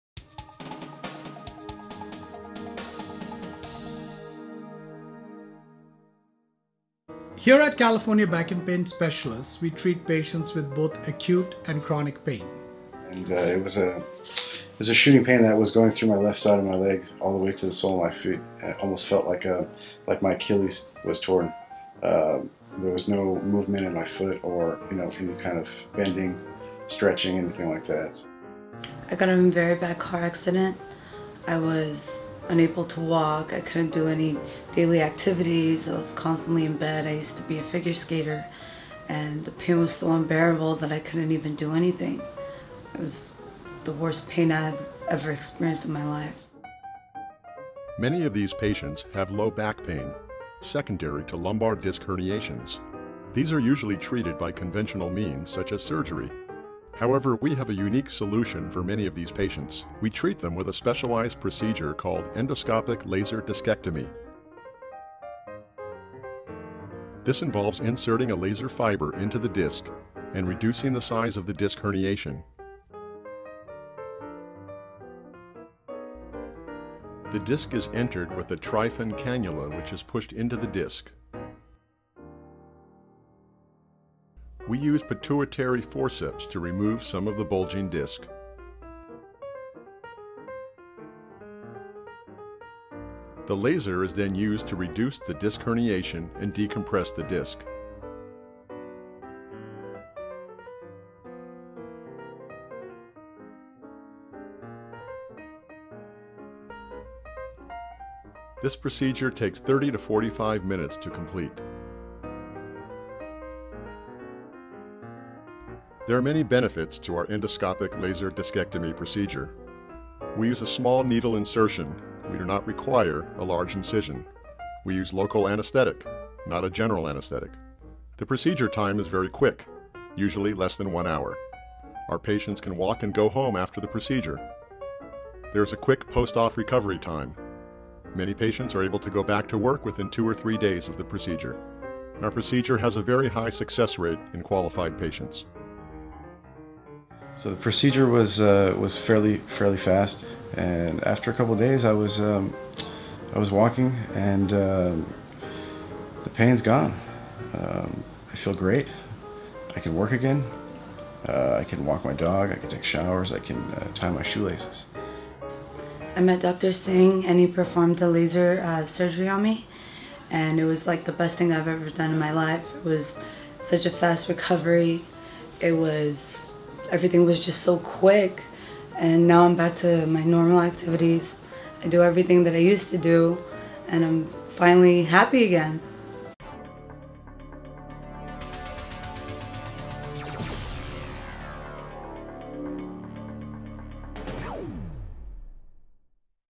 Bakersfield CA Pain Management with Endoscopic Discectomy
Bakersfield pain management with the California Pain Network includes cutting edge procedures such as endoscopic laser discectomy, radiofrequency ablation and spinal cord stimulator implants.
All provide impressive pain relief for back pain, sciatica, neck pain and more.